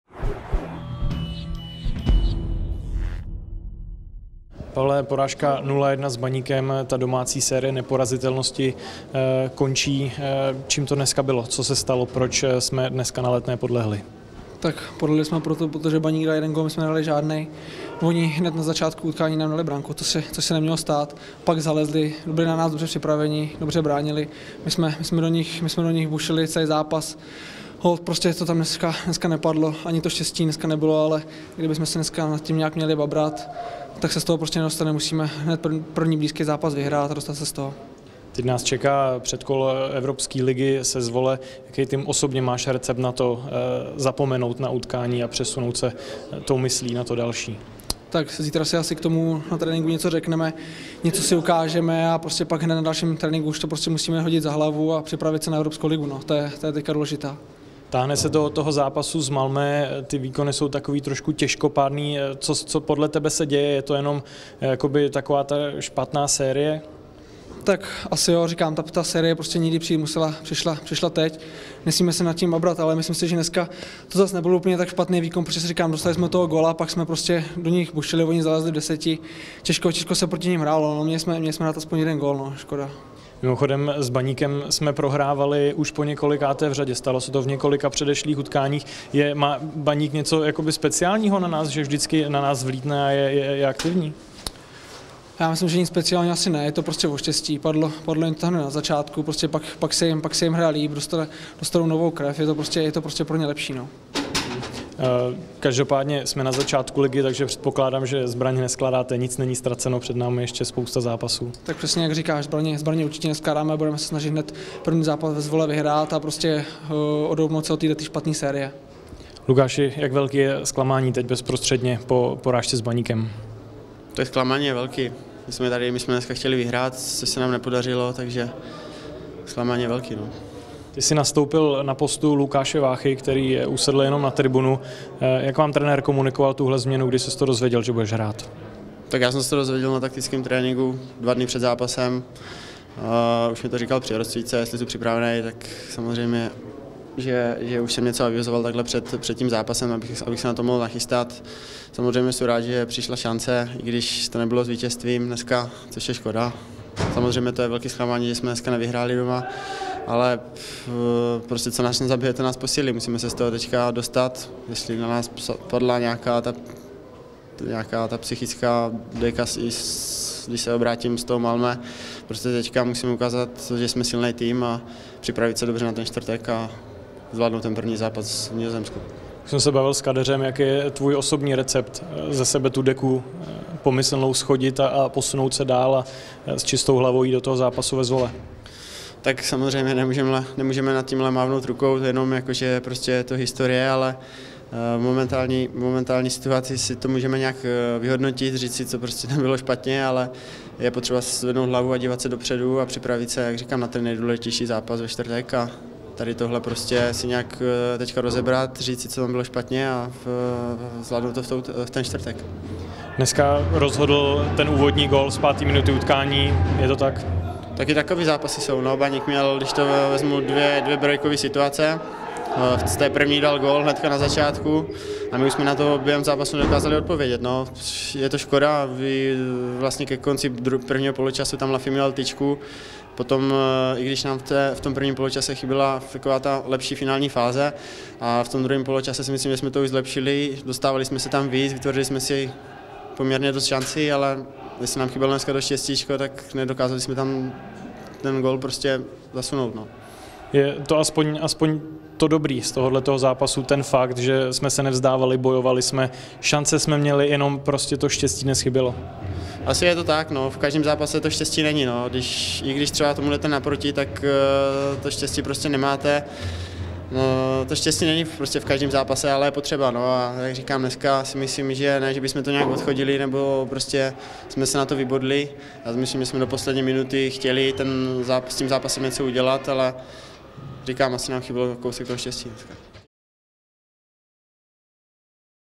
Kadeřábek s Marečkem po porážce s Baníkem
Reakce sparťanských fotbalistů bezprostředně po porážce 0:1 s Baníkem v rámci 4. kola SYNOT Ligy.